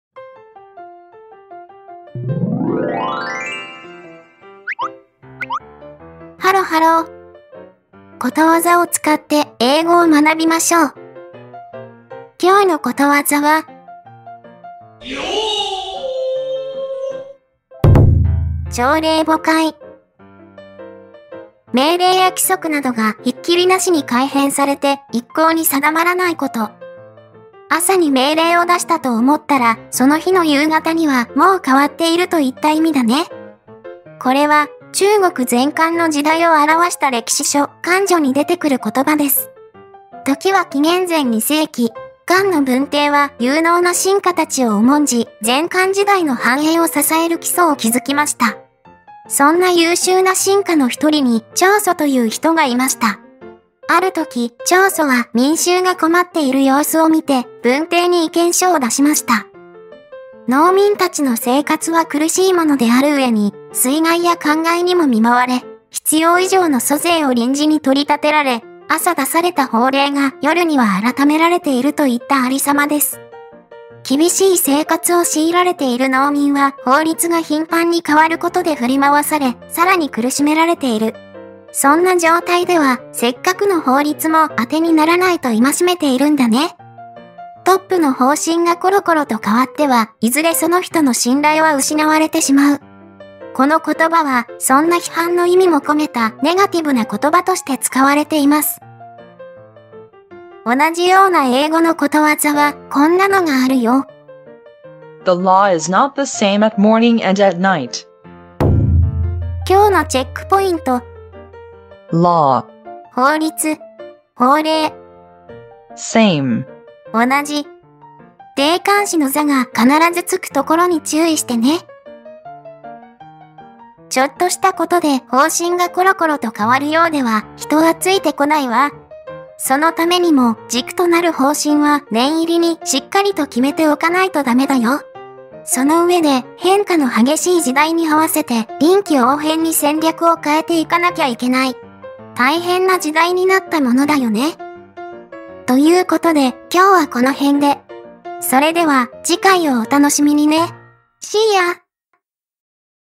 【朝令暮改】ことわざは人生を豊かにするエッセンス！ Let's learn English with Japanese proverbs.
ことわざを通して先人たちの知恵を学ぼう！

■朝令暮改
命令や規則などがひっきりなしに改変されて、いっこうに定まらないこと。
出典：『漢書 - 食貨志』

朝に命令を出したと思ったら、その日の夕方にはもう変わっているといった意味です。
トップの方針がころころと変わっては、いずれその人の信頼は失われてしまう。
この言葉は、そんな批判の意味も込めたネガティブな言葉として使われています。

■英語のことわざ
The law is not the same at morning and at night.
『法律は朝と晩とで同じではない』
・law：法律、法令
・same：同じ（the～）